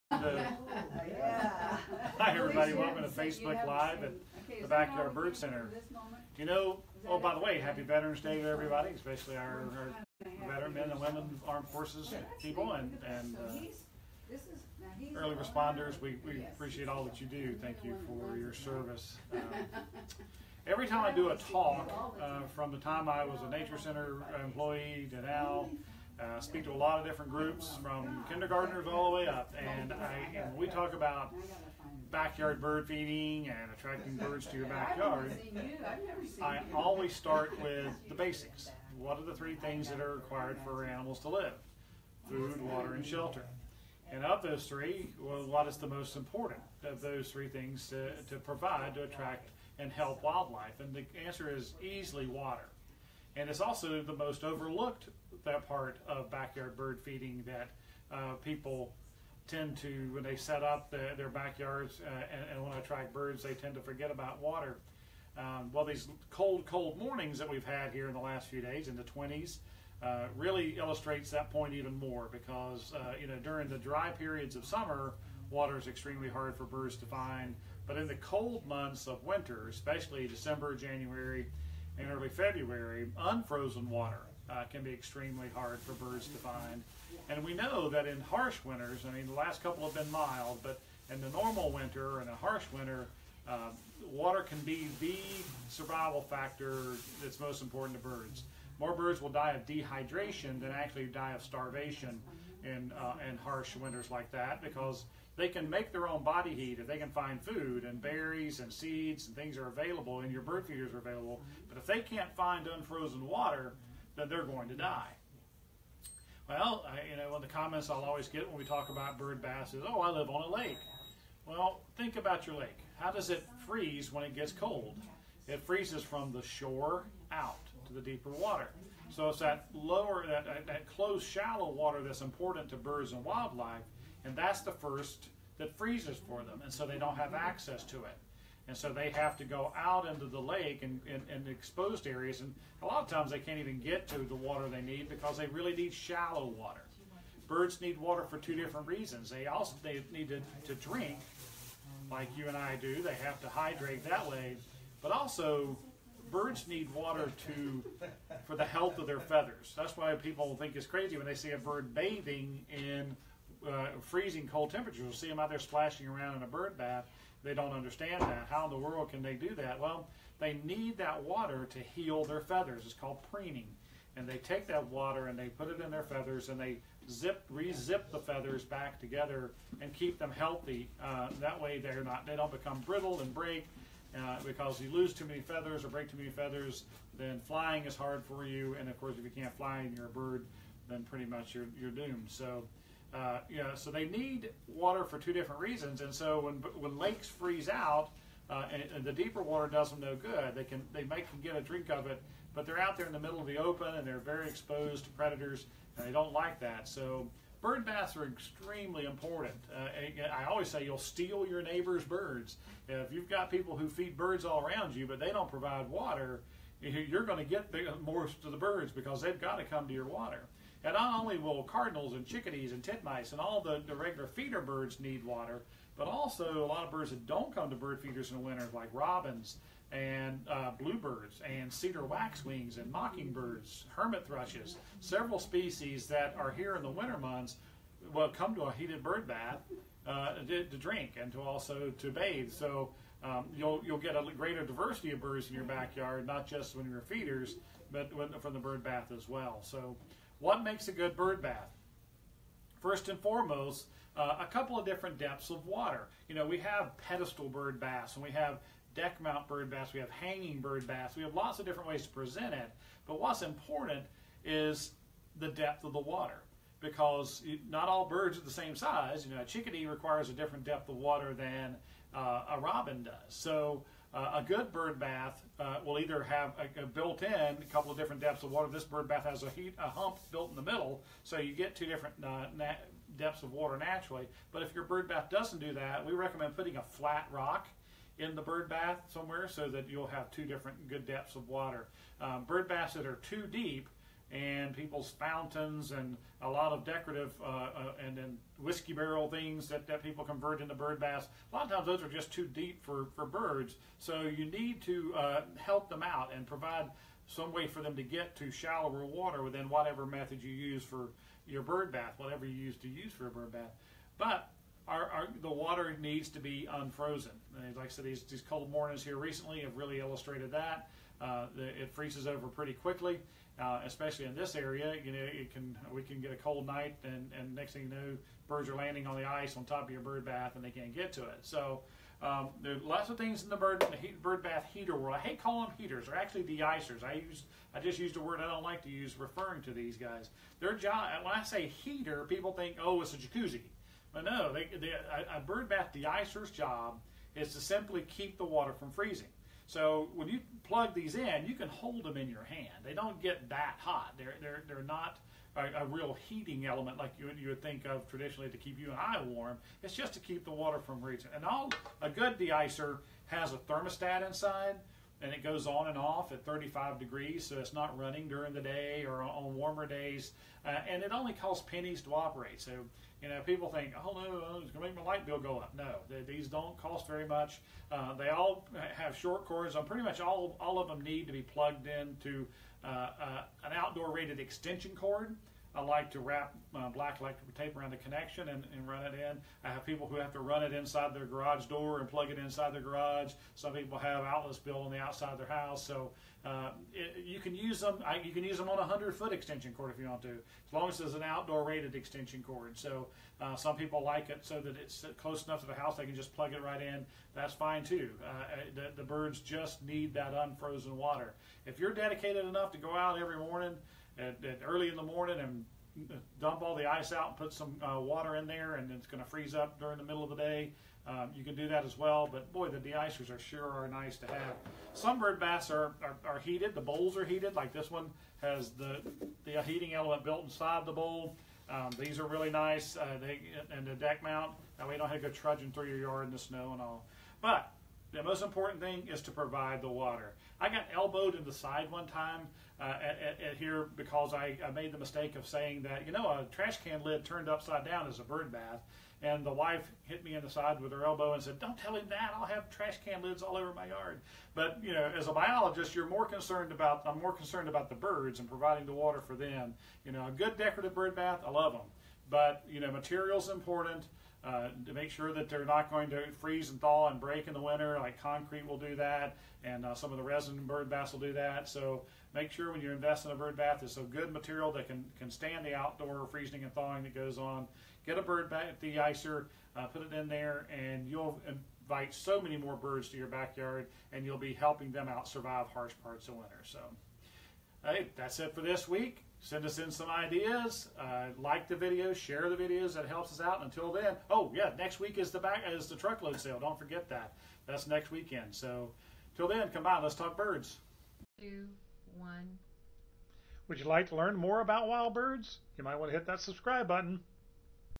Unfrozen Water in Winter is Essential For Birds
A source of clean, unfrozen water is essential for wild birds. Many birds die of dehydration each winter because the cannot find a dependable source of unfrozen water. A good heated bird bath can be the difference between life and death for many birds during harsh stretches of winter. Well maintained bird baths will also greatly enhance the number of birds and the diversity of species you can attract to your yard.